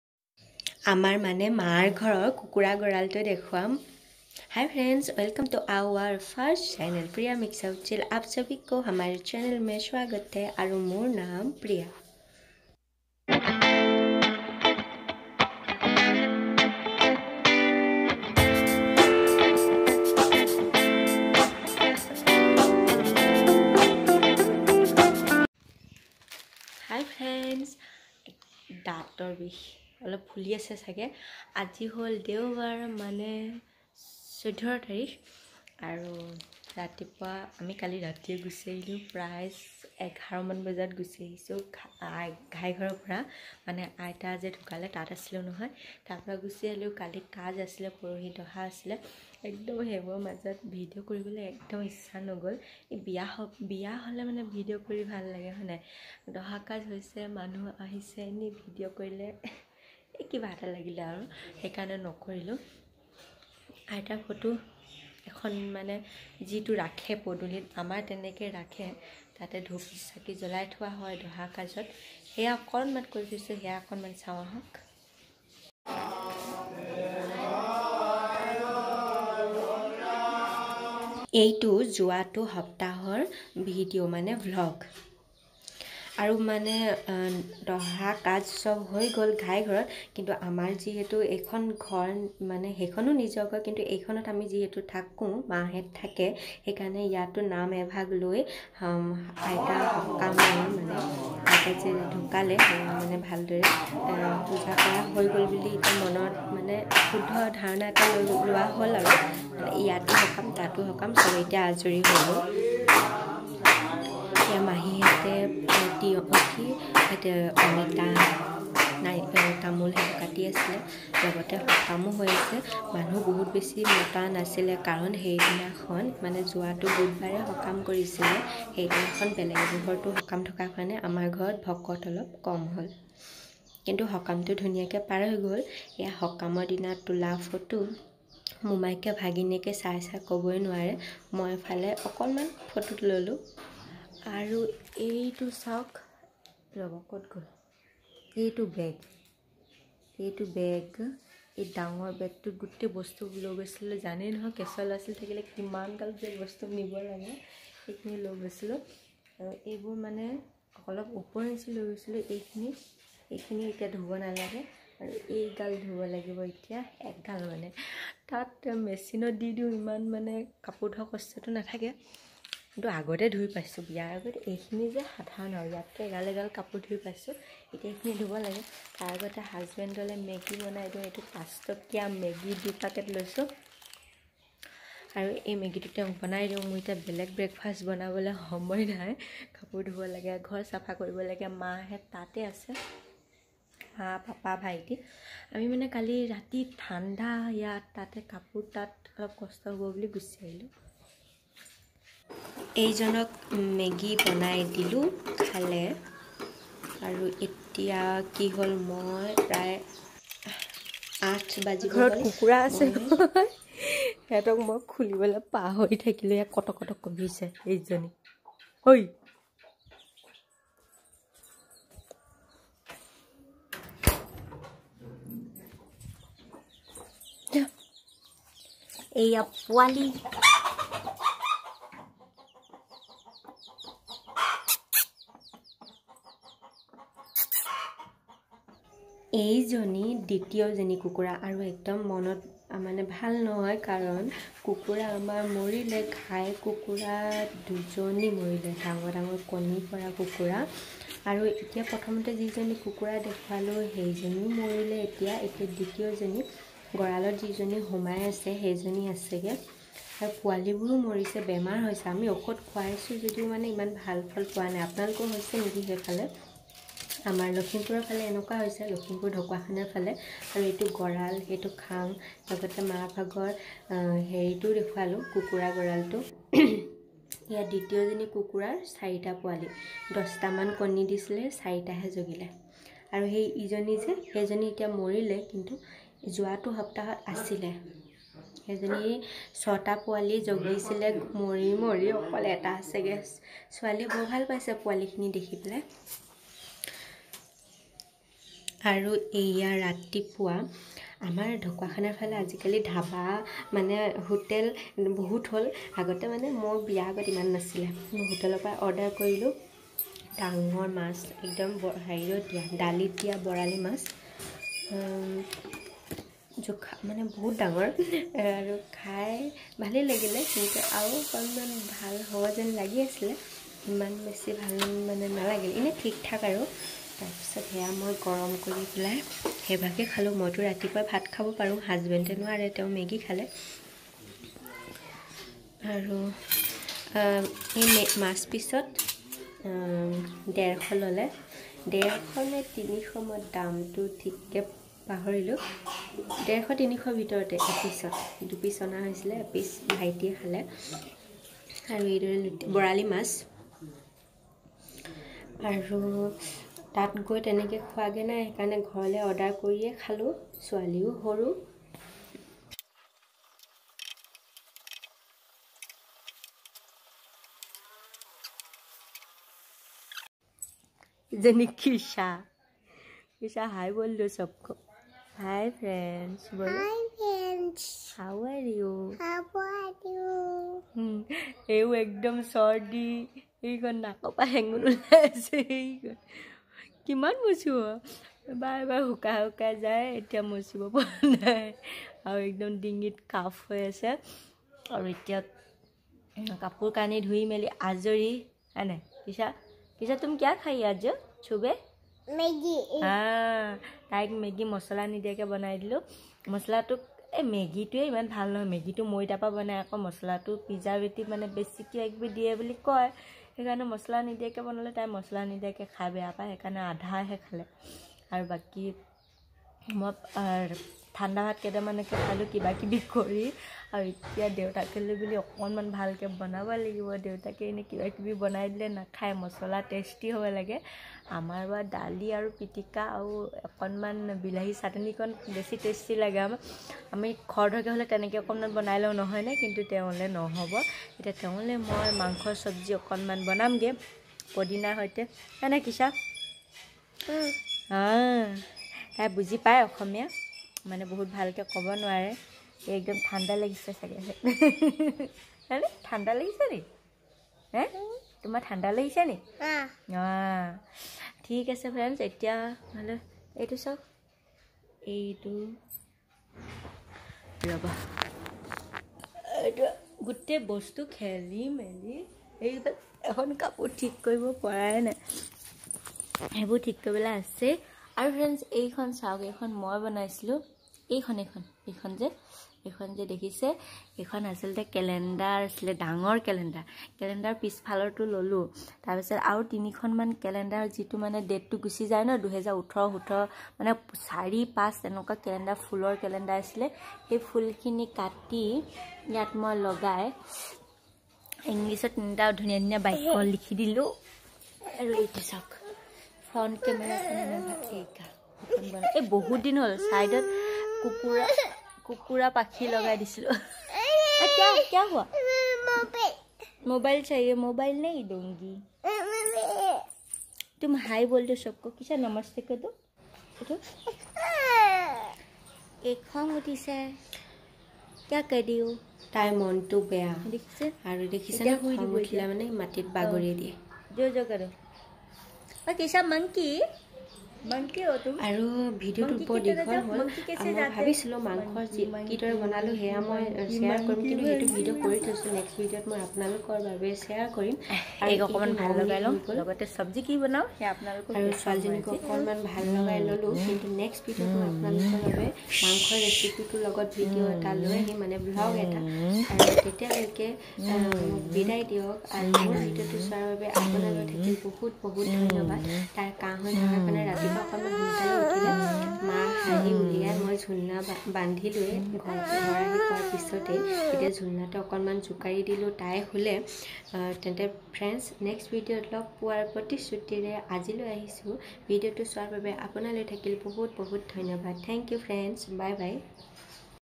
ডেৰ বছৰৰ মূৰত মাৰ ঘৰলৈ আহি এক পিচ বৰালি মাছত 150 টকাৰে -- গড়াল খুলিব থাকি গল -- Priya Mixture Uchil
Hii Friends
Welcome to our 1st channel Priya Mixture Uchil